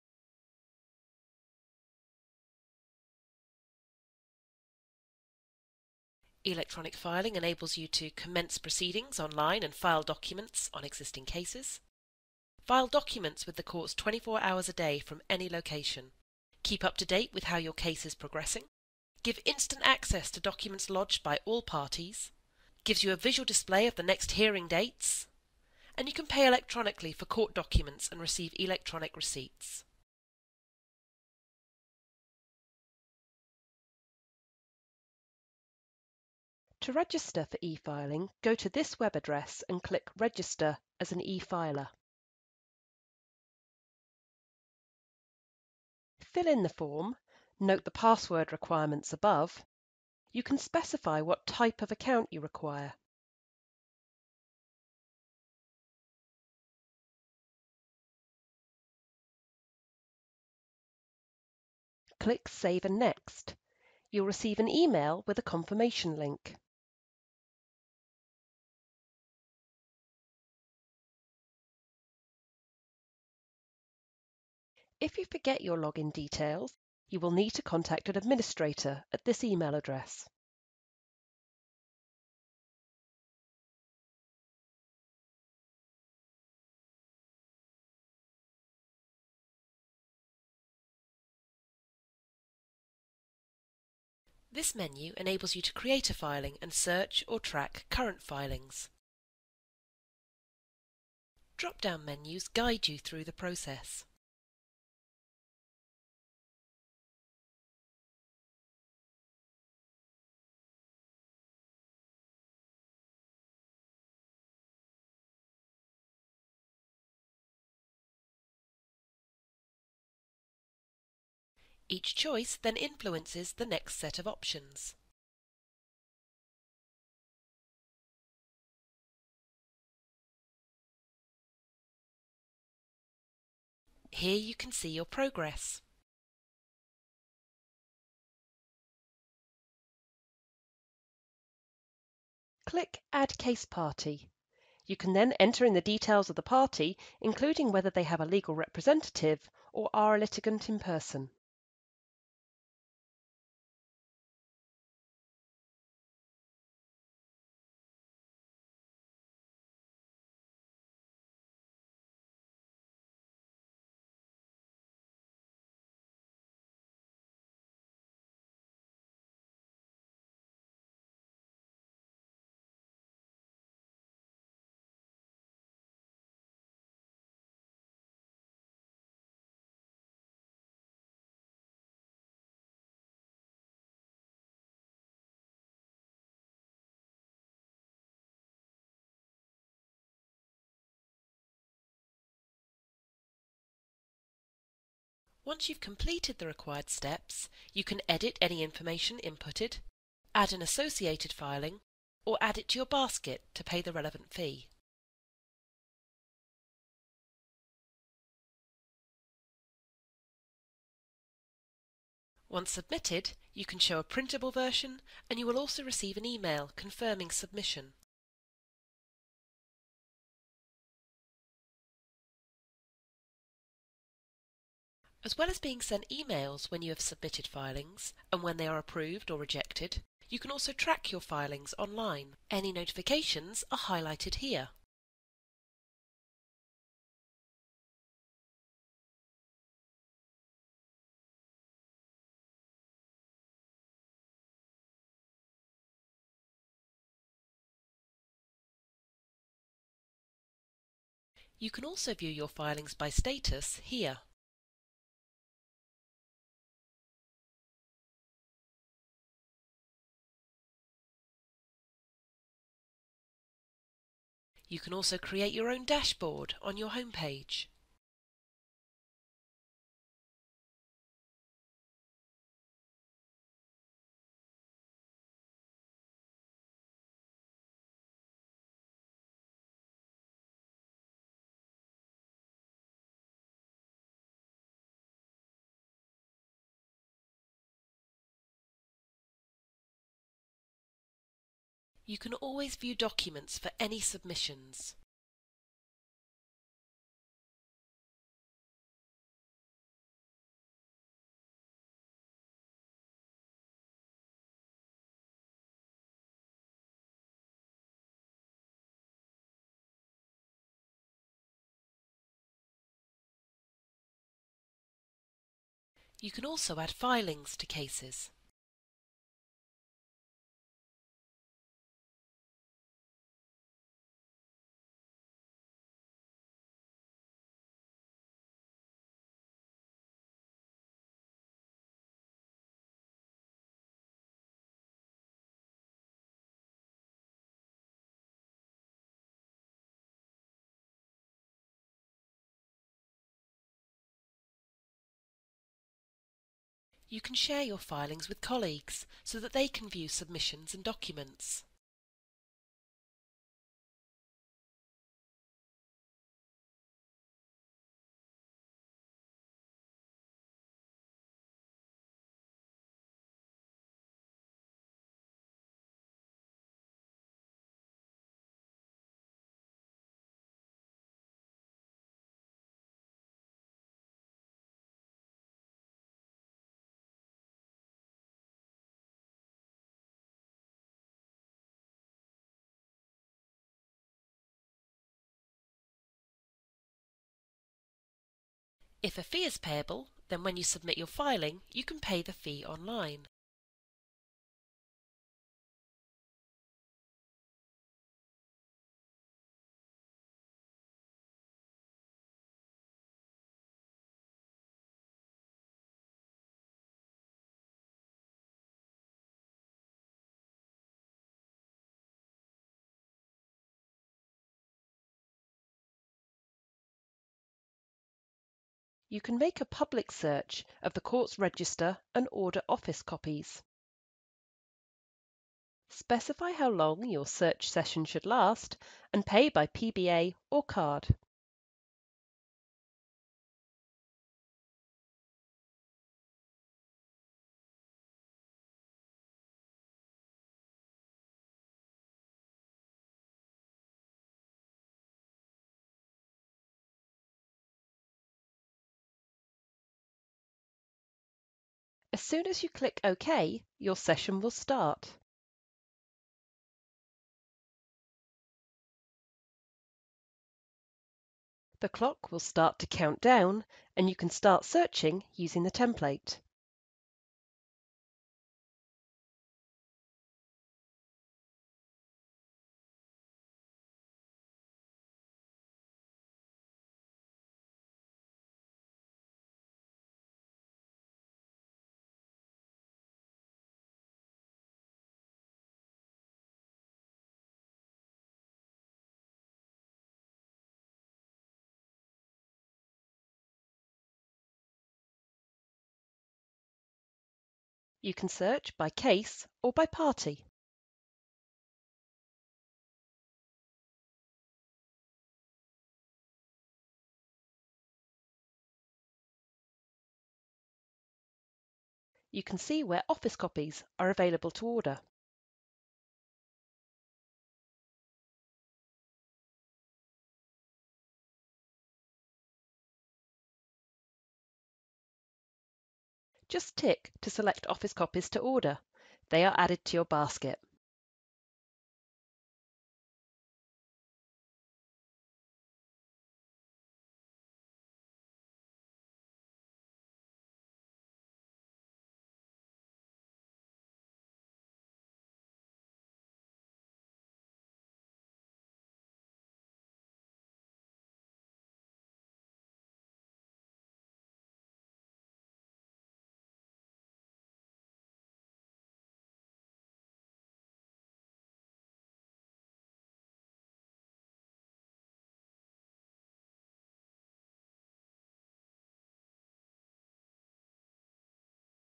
Electronic Filing
Slide 1:
Electronic Filing enables you to:
- Commence proceedings online and file documents on existing cases
- File documents with the court 24 hours a day from any location
- Keep up to date with how your case is progressing
- Instant access to documents lodged by all parties
- Visual display of the next hearing dates are displayed on your cases
- Pay electronically for court documents and receive electronic receipts

Slide 2:
Registering for efiling

Slide 3:
To register for efiling go to this web address Click Register as an E-Filer.

Fill in the form (note the password requirements above).
You can specify what type of account you require.

Click Save & Next.
You will receive an email with a confirmation link.

Slide 4:
Creating a Filing.

Slide 5:
This menu enables you to create a filing and search or track current filings.

Drop-down menus guide you through the process.

Each choice then influences the next set of options
Here you can see your progress.

Click Add Case Party.

You can then enter in the details of the party including whether they have a legal representative or are a litigant in person.

Once you have completed the required steps, you can edit any information inputted, add an associated filing or add it to your basket to pay the relevant fee.

Once submitted, you can show a printable version and you will also receive an email confirming submission.

Slide 6:
Tracking your Filings.

Slide 7:
As well as being sent emails when you have submitted filings and when they are approved/rejected, you can also track your filings online.

Notifications are highlighted here. You can also view your filings by status here. You can also create your own dashboard on your Home page.

Slide 9:
Viewing document and adding filings.

Slide 10:
You can always view documents for any submissions.

You can also add Filings to Cases.

Slide 11:
Sharing with colleagues.

Slide 12:
You can share your Filings with colleagues so that they can view submissions and documents.

Slide 13:
Online payment.

Slide 14:
If a fee is payable then when you submit your filing you can pay the Fee online.

Slide 15:
Public Search,

Slide 16:
You can make a public search of the courts’ register and order office copies

Specify how long your search session should last and pay by PBA or card.

As soon as you click OK, your session will start.

The clock will start to count down and you can start searching using the template.

Slide 17:
You can search by Case or by Party

You can see where office copies are available to order.

Just tick to select office copies to order - they are added to your basket.